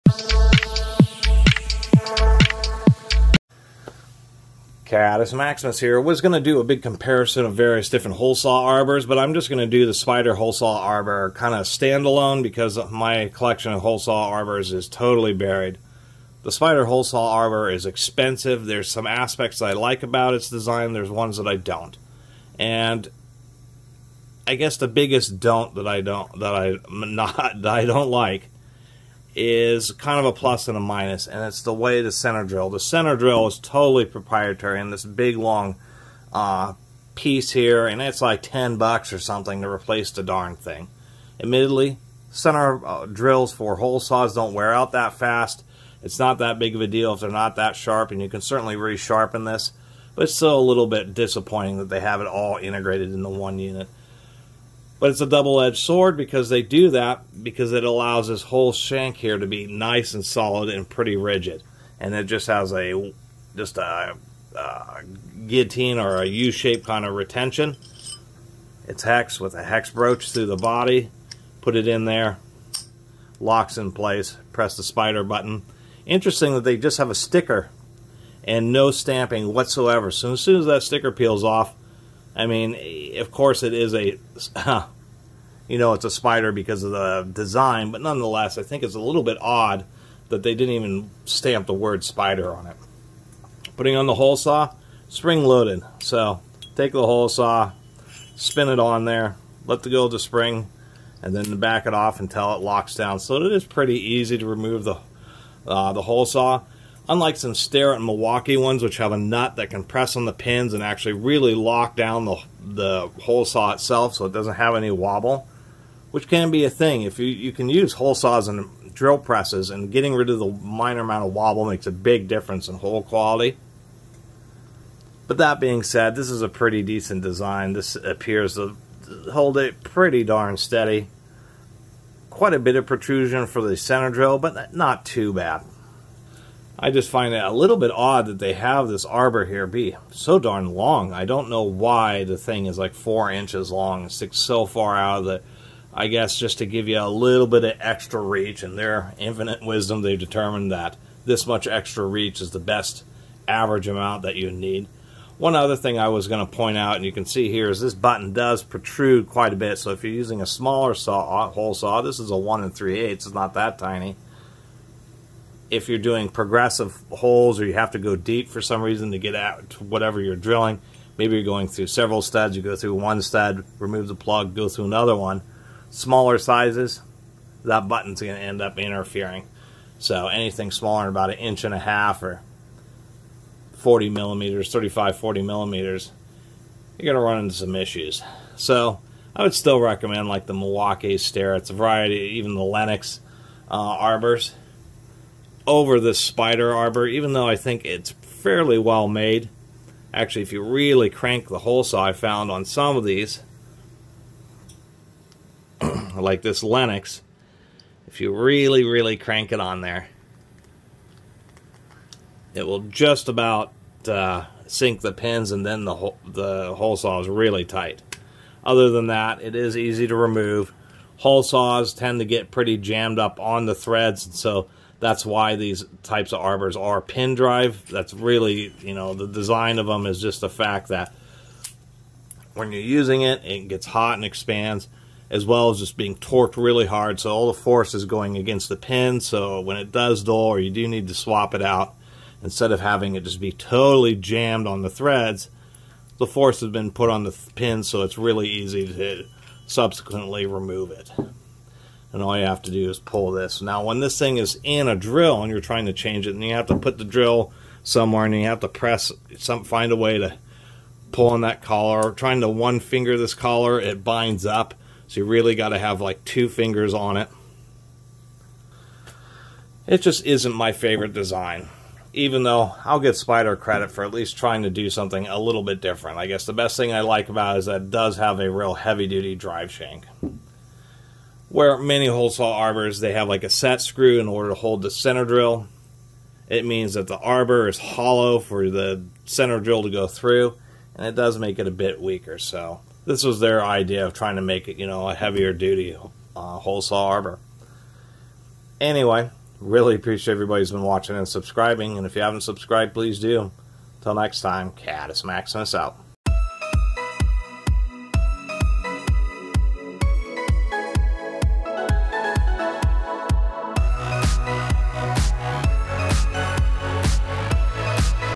Spyder Hole Saw Arbor Review
This is a expensive armor that uses special center drills but it does give it a strong heavy duty drive shank that is less likely to bend like the hollow ones of standard arbors, to bad they did make a way to replace the pilot drill without replacing the expensive assembly.